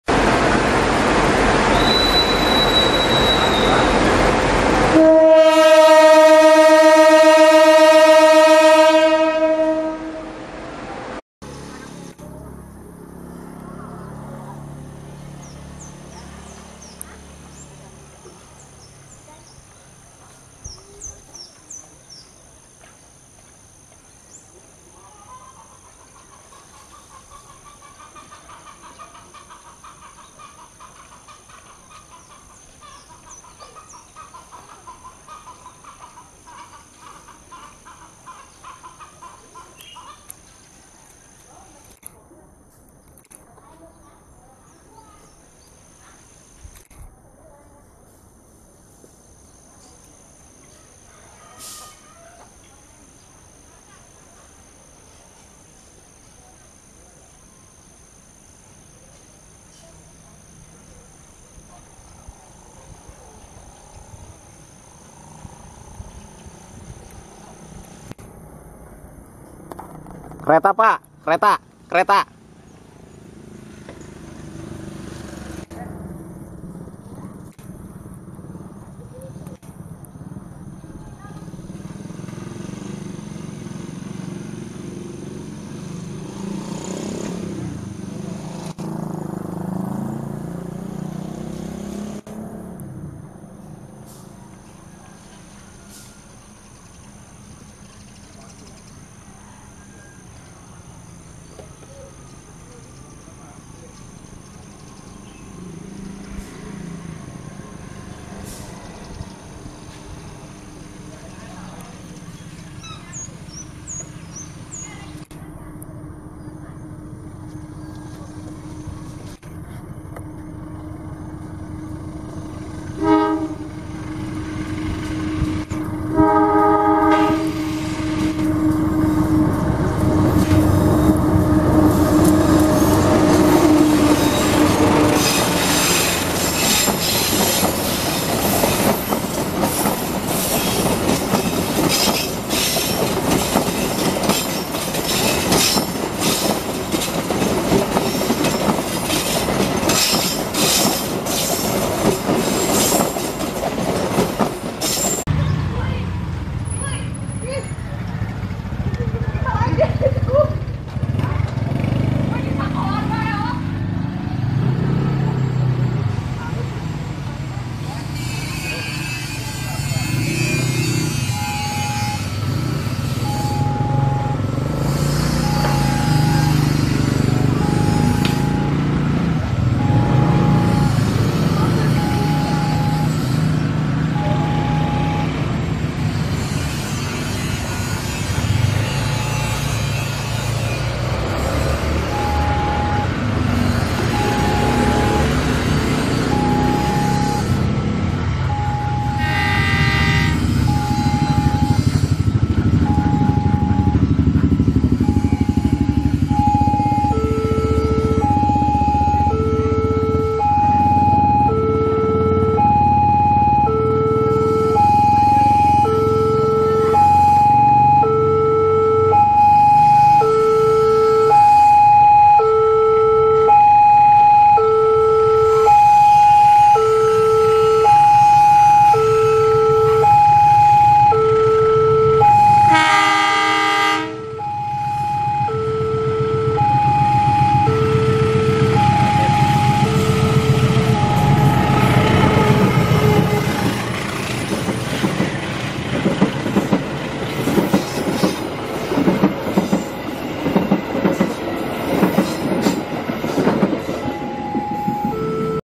Ada Kereta!! Usahakan Tengok Kanan Kiri
Seringnya terjadinya kecelakaan tertemper kereta api diakibatkan oleh diri nya sendiri, seperti inilah contoh ketika akan melewati perlintasan liar (efek jalan mulus beton) tidak memperhatikan sisi kanan kiri nya terfokus kedepan menatap jalanan..

selain itu juga ini adalah video perbandingan antara tahun 2018 & 2021 dimana ketika tempat ini belum diberi palang pintu perlintasan dan langganan tertemper kereta api dan juga ketika rel ini di upgrade.